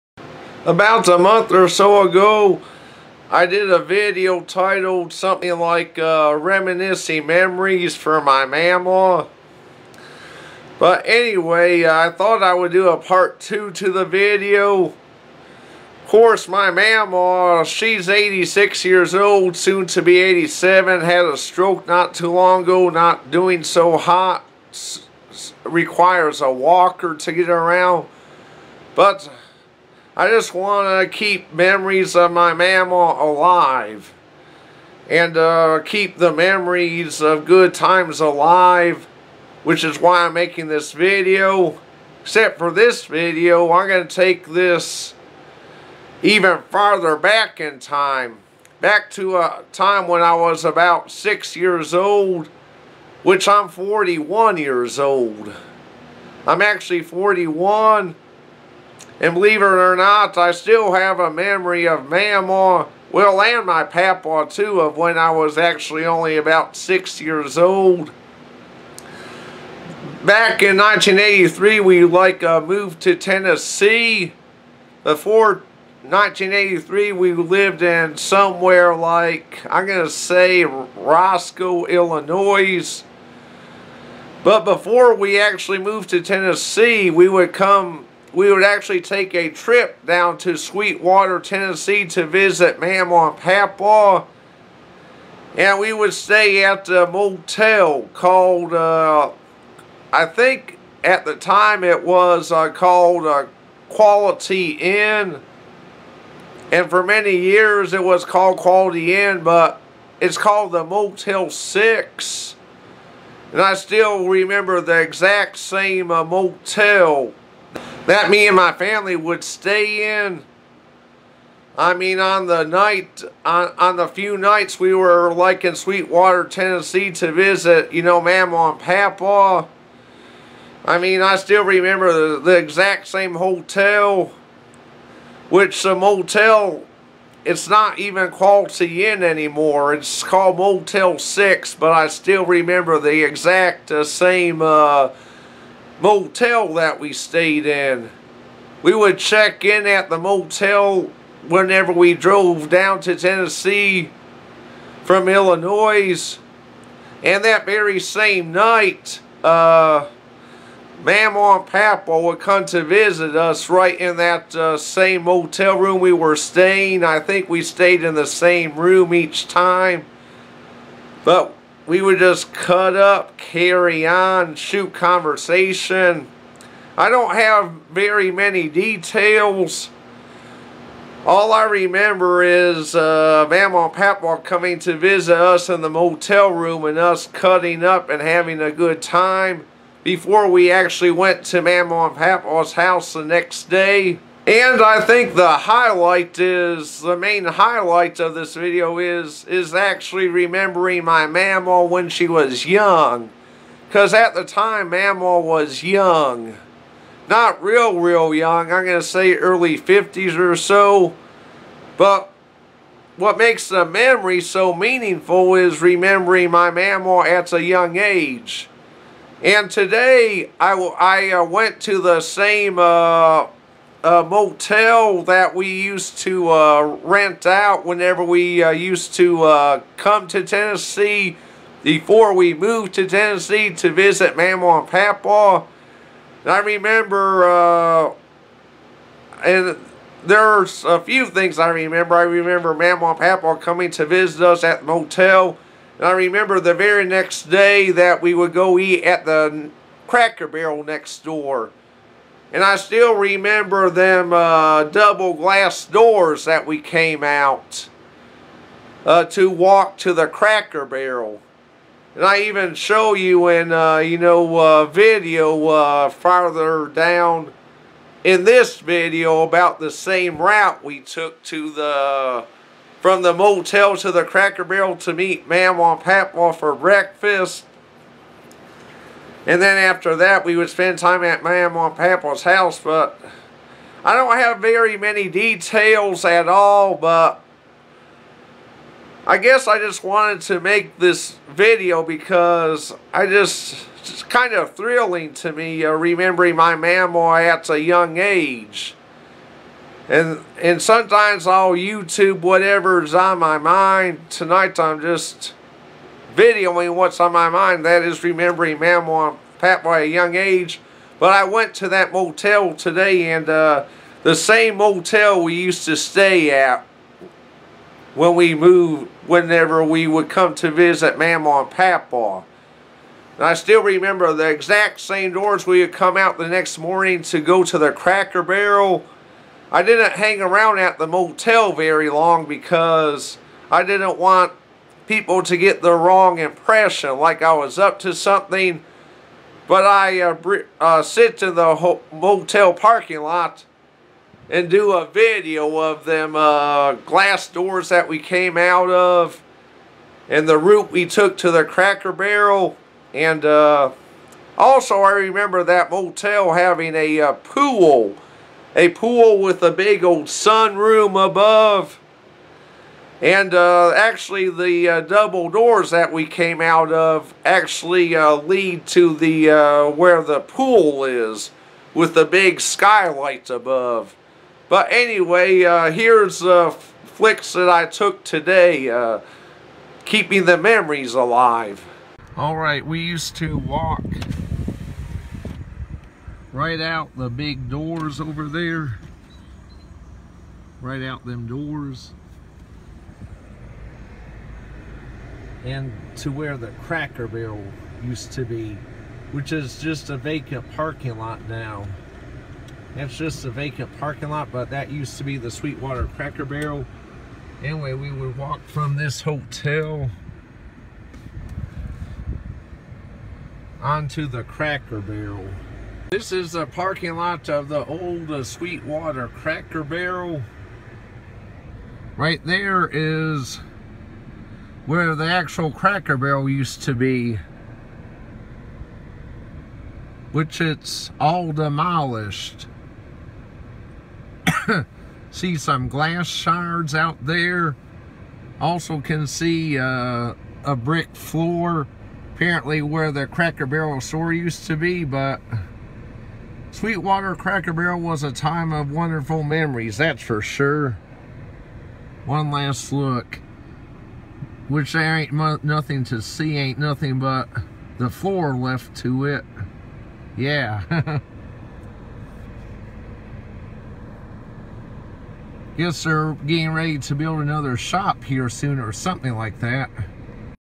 Reminiscing Memories For Mamaw, Part 2
Here's a Part 2 video continuing from "Reminiscing Memories For Mamaw."  I take it back to an even earlier time when I was about 6 years old.  I even remember the motel that we used to stay at.